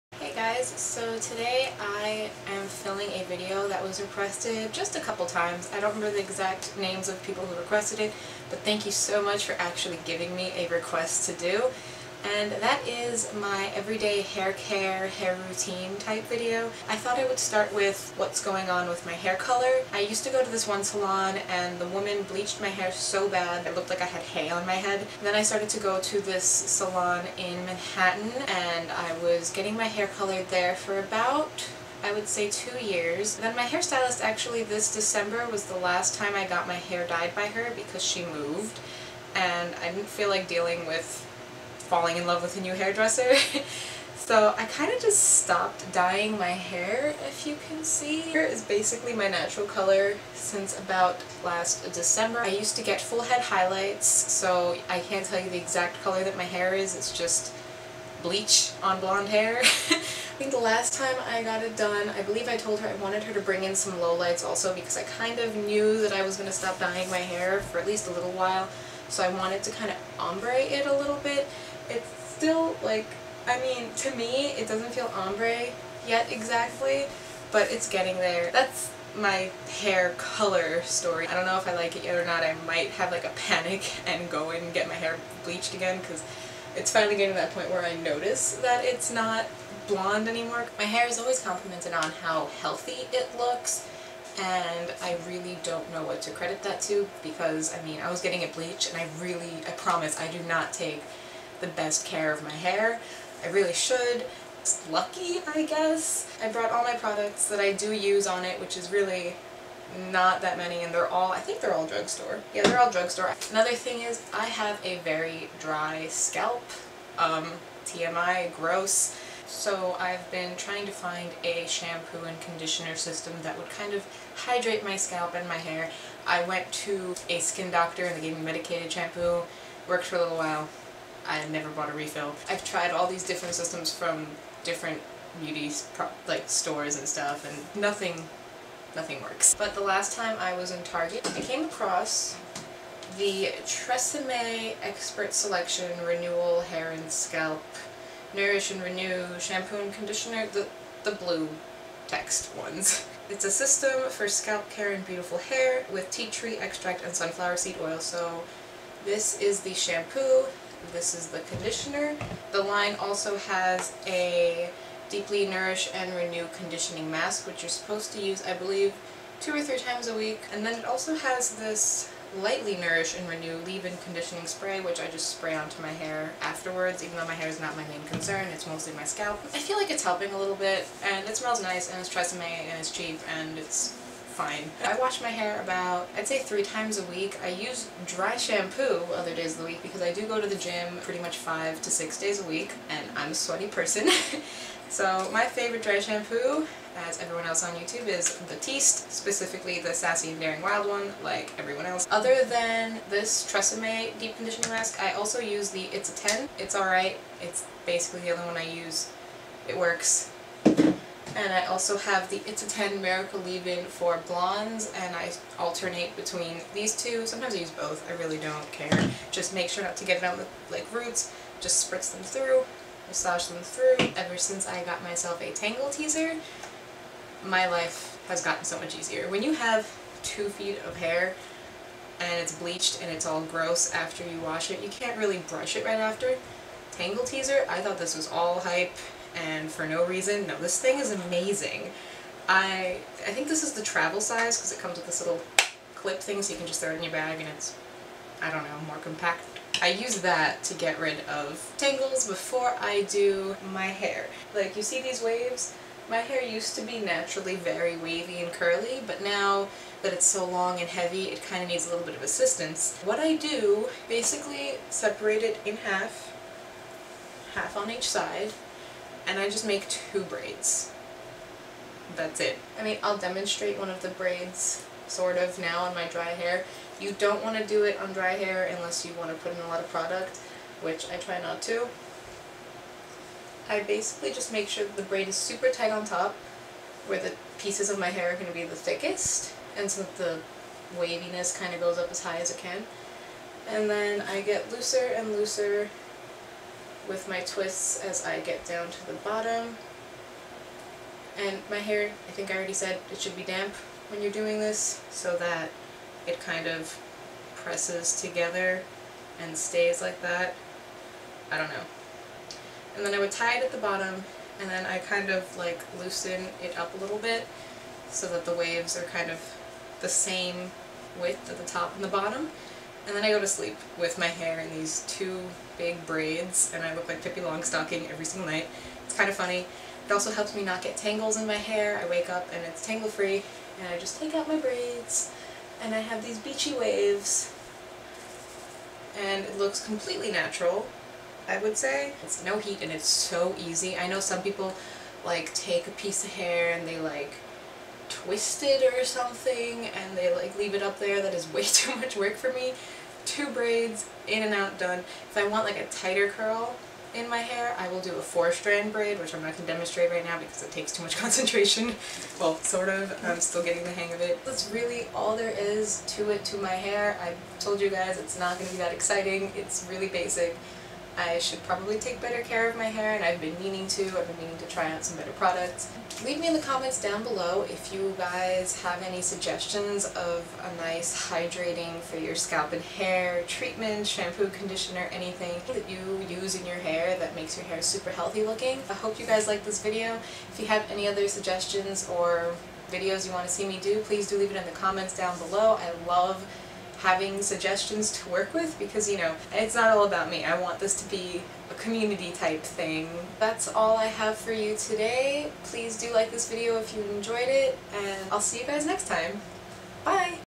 PERFECT SUPER EASY NO HEAT CURLS & HAIR CARE ROUTINE!
Hey guys! Hope you enjoy this hair video! I covered bases from what my hair color is, what products I use, and how I get my beachy waves everyday.

When shopping online, use my ebates link to get ca$h back!

Get some of my favorite perfumes at Notino!

All thoughts and opinions are my own, and all items mentioned in the video were purchased with my own money!